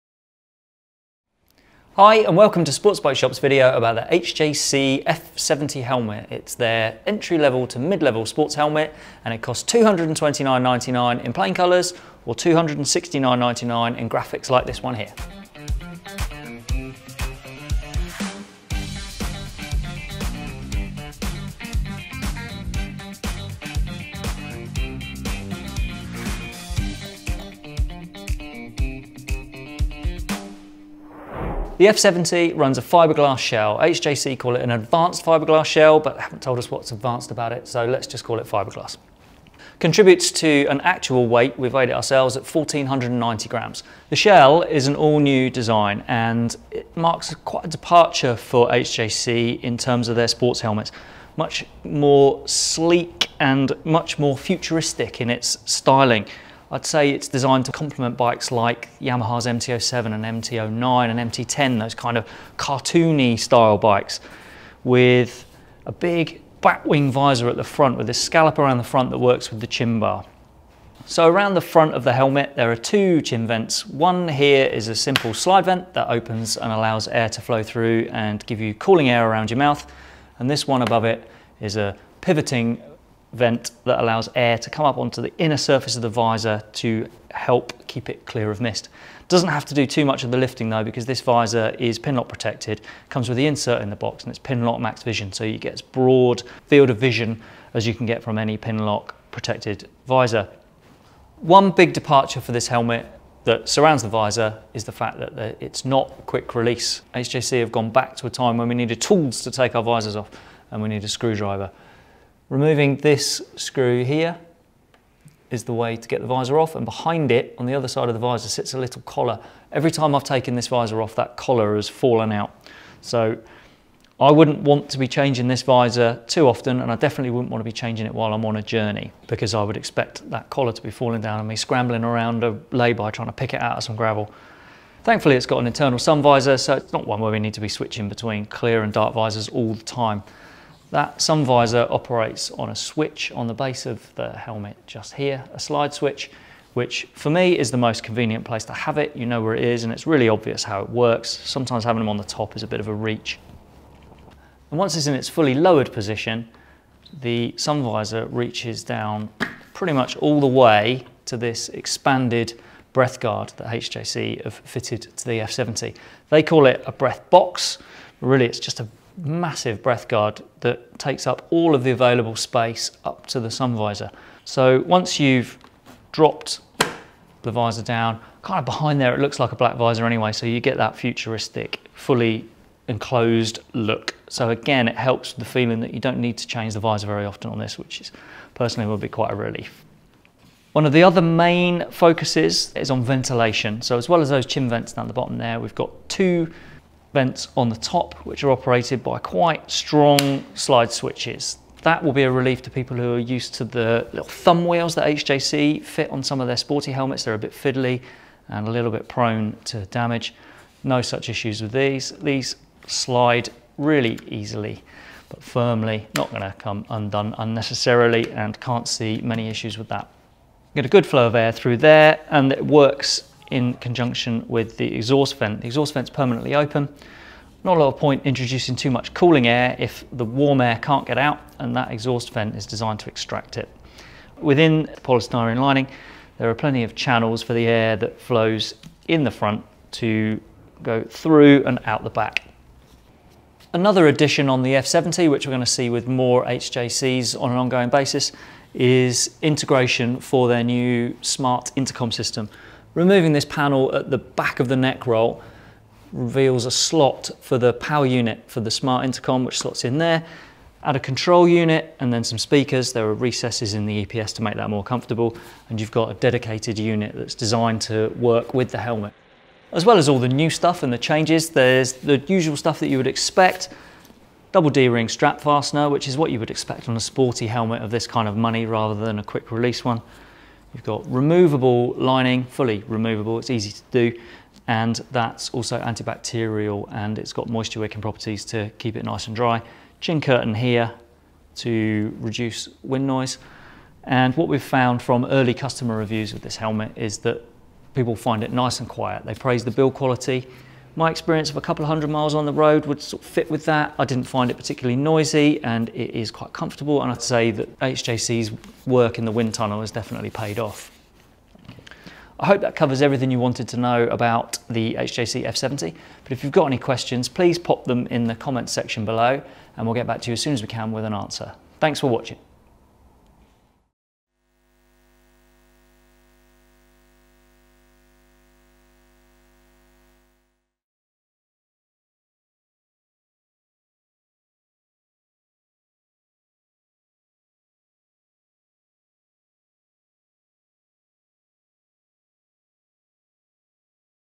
HJC F70 motorcycle helmet review - Sportsbikeshop
HJC have gone their own way with the futuristic styling on the F70 helmet. It’s sporty, looks cool, has good vents and a mad breathguard. Want to know ALL about it? Watch this...